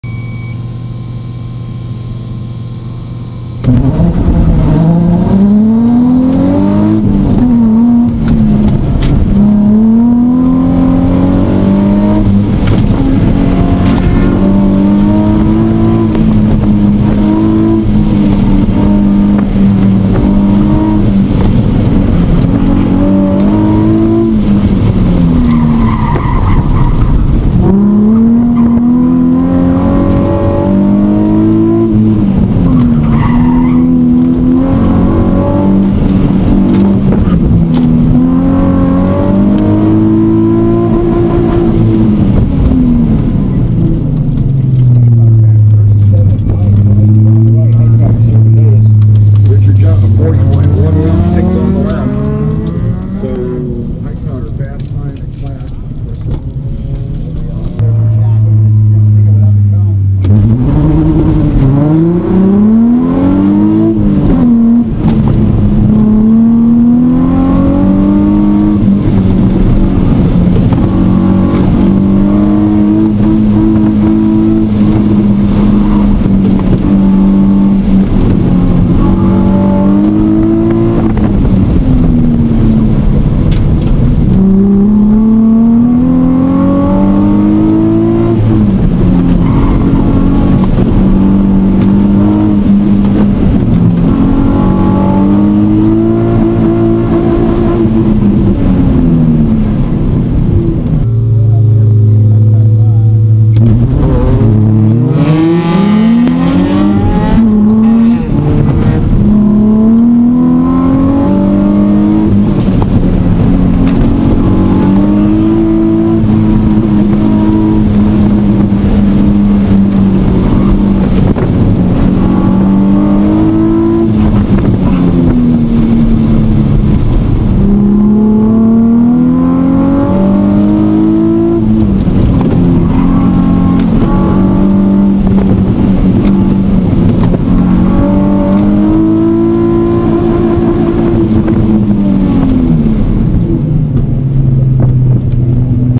2016 El Toro ProSolo STU 350Z
First time on Bridgestone RE71-Rs in 9 months on this car.

Handling not ideal on these tires, because they've added so much grip compared to what the car was set up with, but it's still good enough to be fast if you find the sweet spot managing the car in transitions.

My fastest clean right side run was a 37.659, so I really didn't do my dial-in any favors! I also had a 37.446(+1) and a 37.583(+1) on that side. and it was the same slalom cone every time!

First run of the weekend (at the start of this video) was a 37.990.